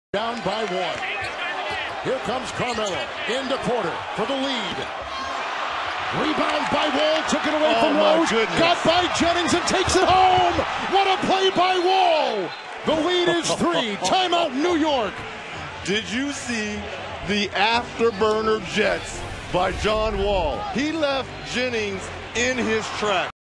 Wall goes the distance for game-sealing dunk
After Carmelo Anthony misses a go-ahead shot, John Wall gets the rebound and turns on the jets to connect on the difference maker in the Wizards' 113-110 win over the Knicks.